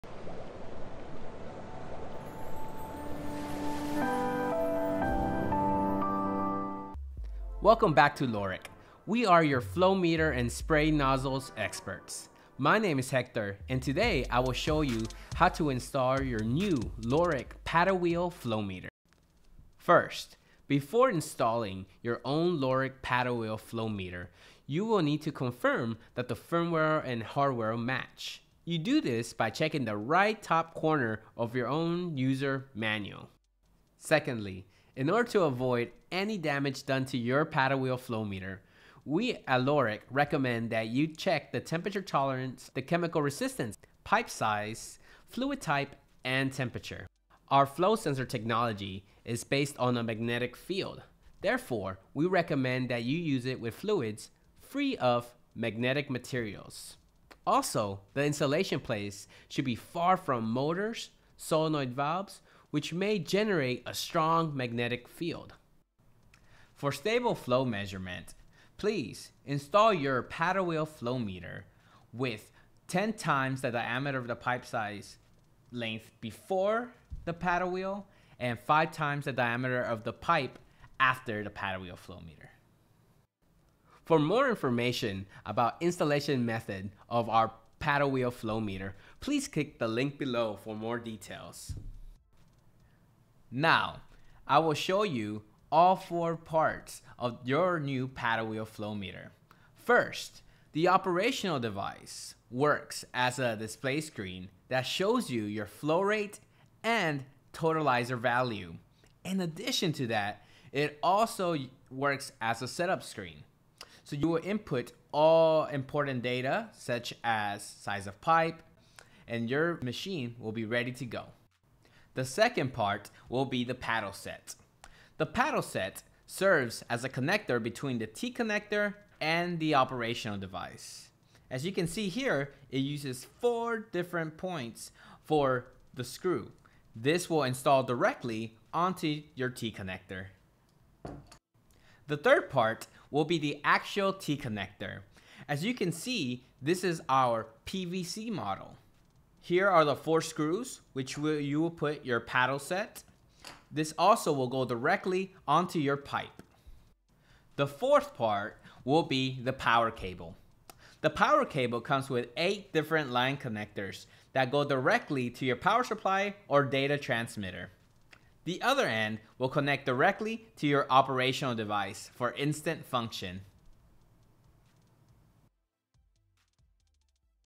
LORRIC FP-AS510 Paddle Wheel Flowmeter Product Introduction
This video is about LORRIC FP-AS510 Paddle Wheel Flowmeter Product Introduction
Section:
-Matters needing attention
-Each parts of Paddle Wheel Flowmeter shots